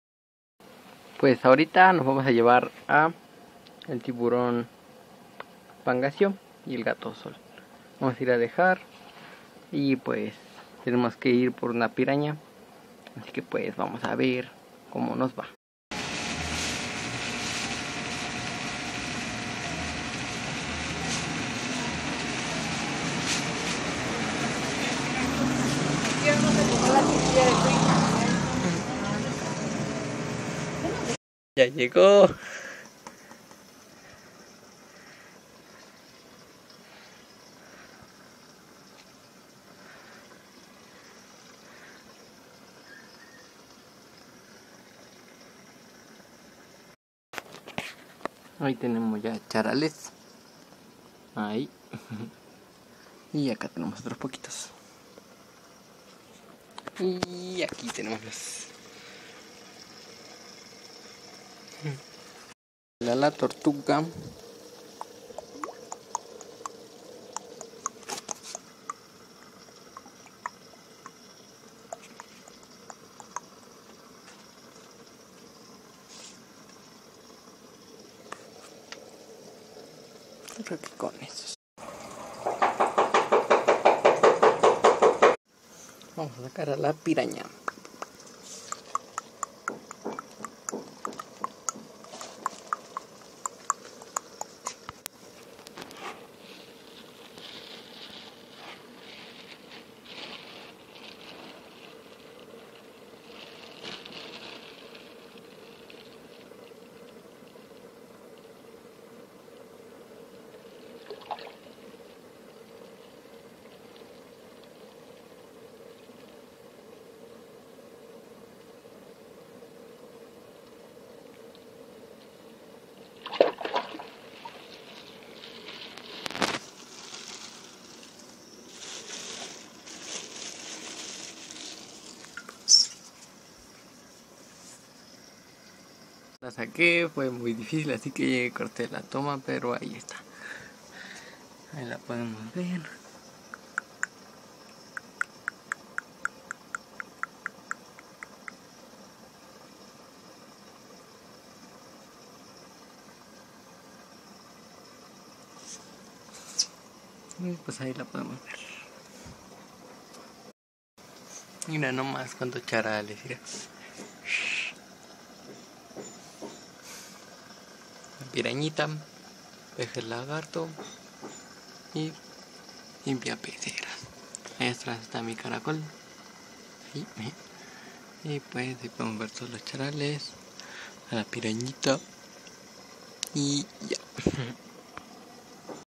Vendo mi tiburón pangacio y mi gato sol y compro una piraña
Vendo mi tiburón pangacio y mi gato sol y compro una piraña y muchos charales para que coman los pejelagartos ,la piraña y la tortuga